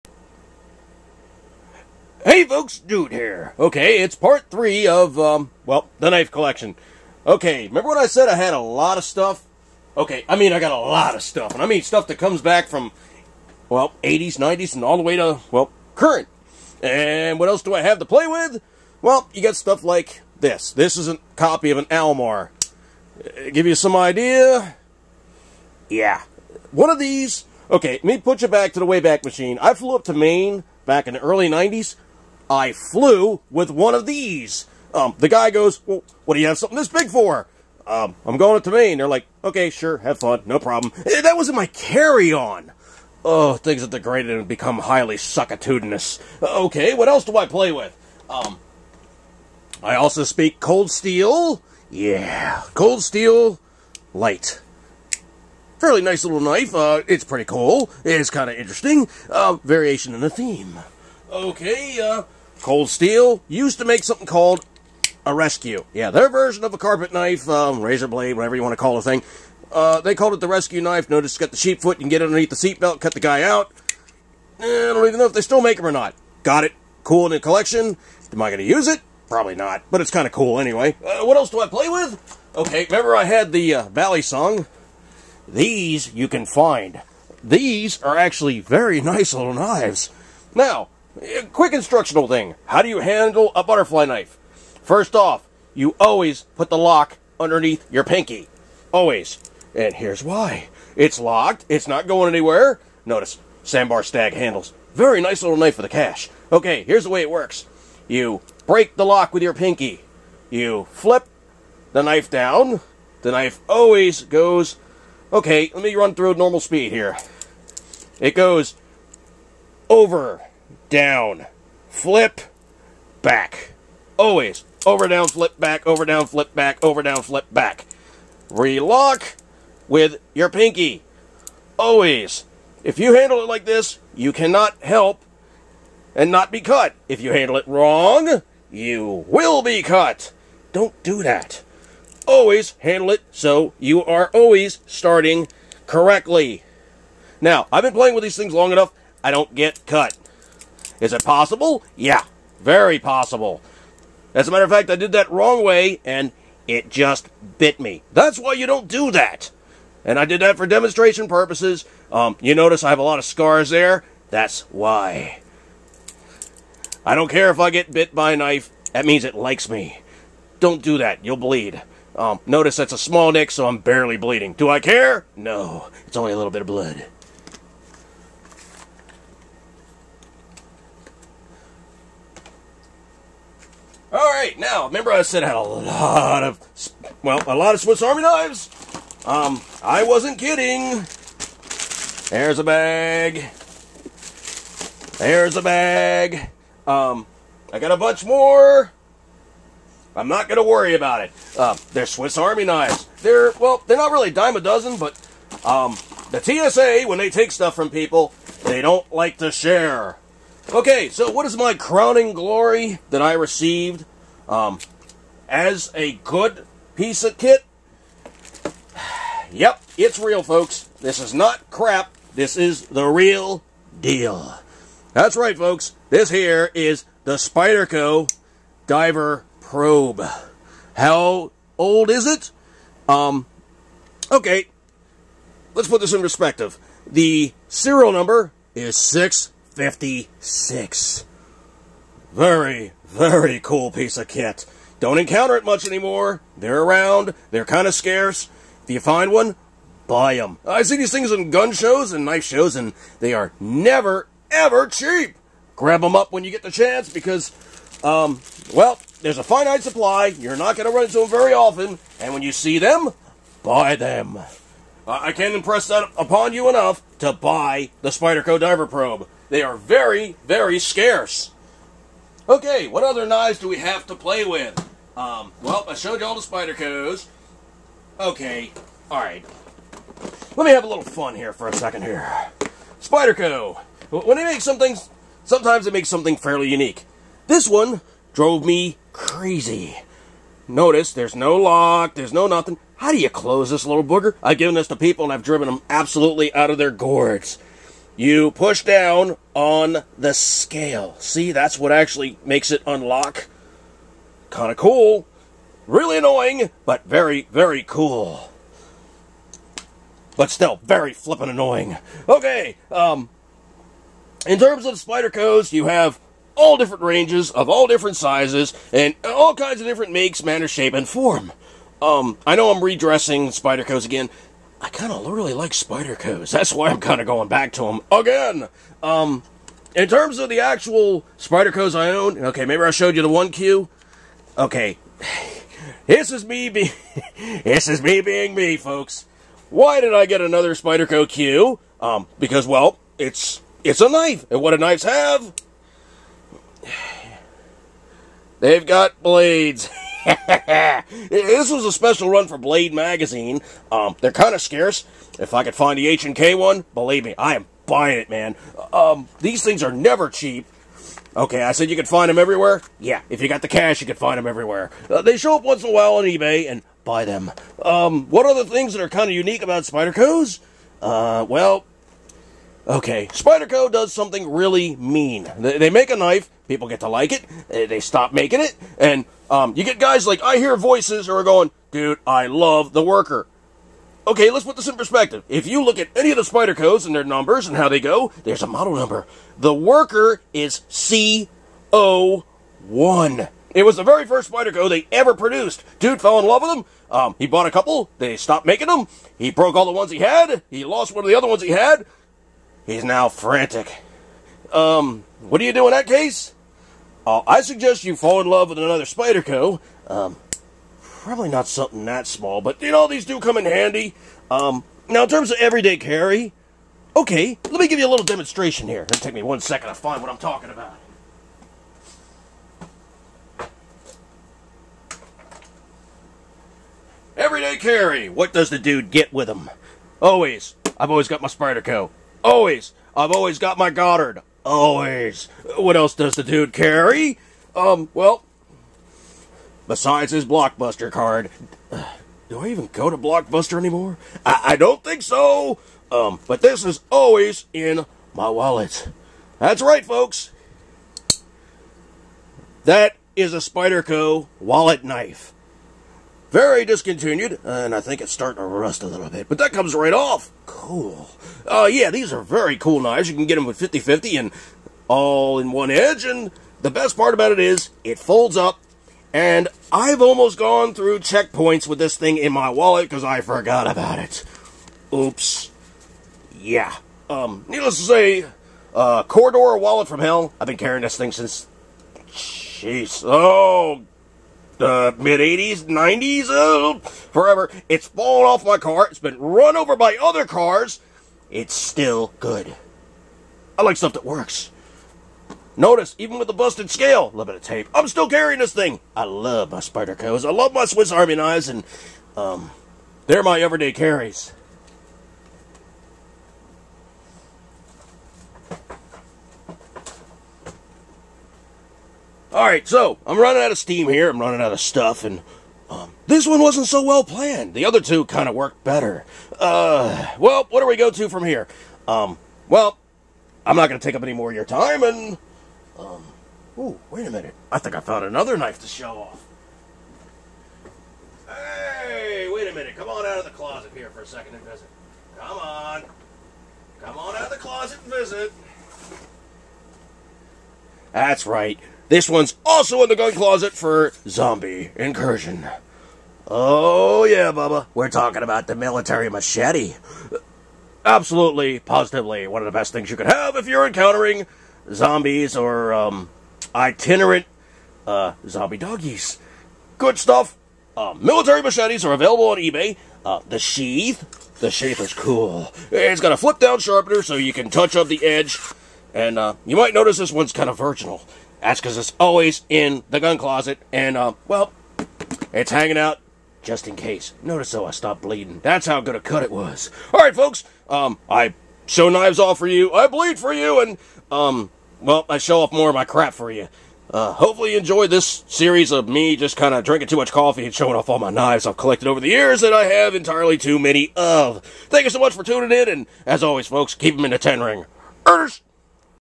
Knife Collection PT3
here's the final chapter on my knife collection video..
also there's some additional content..
note..NOT FOR THE SQUEEMISH
I get bit by one of my butterfy knives..it likes me
meh, it's only a scratch